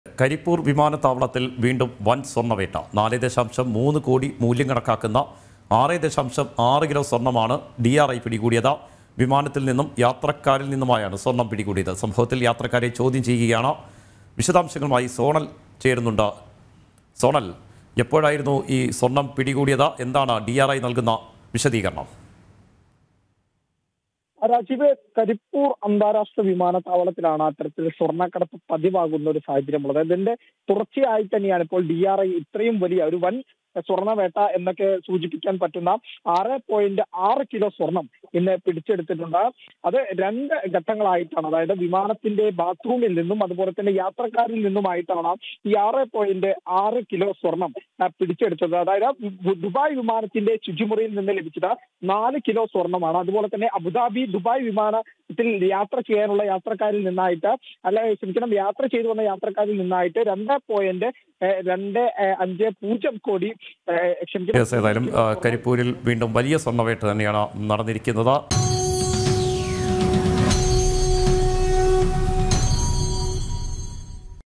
കരിപ്പൂർ വിമാനത്താവളത്തിൽ വീണ്ടും വൻ സ്വർണവേട്ട | KARIPUR | GOLD HUNT
കരിപ്പൂർ വിമാനത്താവളത്തിൽ വീണ്ടും വൻ സ്വർണ്ണ വേട്ട. 4.3 കോടി മൂല്യം കണക്കാക്കുന്ന 6.6 kg സ്വർണമാണ് DRI പിടികൂടിയത്. വിമാനത്തിൽ നിന്നും, യാത്രക്കാരിൽ നിന്നുമായാണ് സ്വർണം പിടികൂടിയത്. സംഭവത്തിൽ യാത്രക്കാരെ ചോദ്യം ചെയ്യുകയാണ്.

NEWS ANCHOR : R RAJIVE